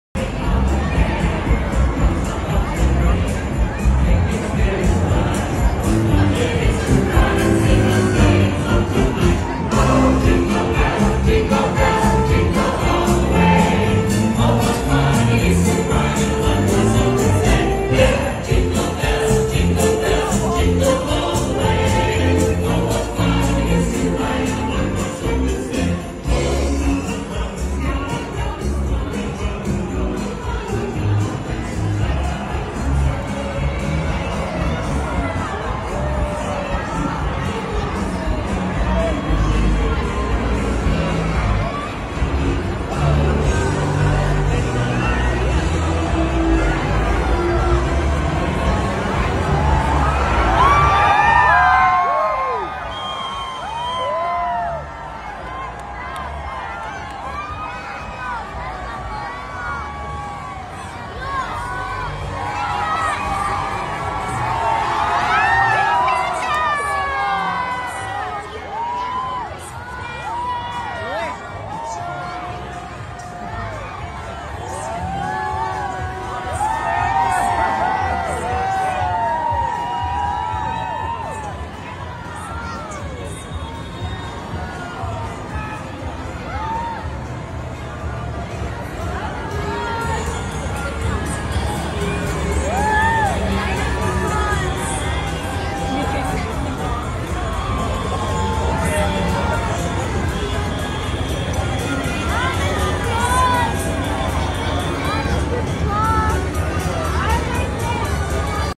Christmas Opening Manhattan, New York 🎄🎄🎅
Shorts
YTShorts
YoutubeShorts

Who doesn't love Christmas in New York? Wondering what to do in NYC during the holidays we've got you covered from the Rockettes to Xmas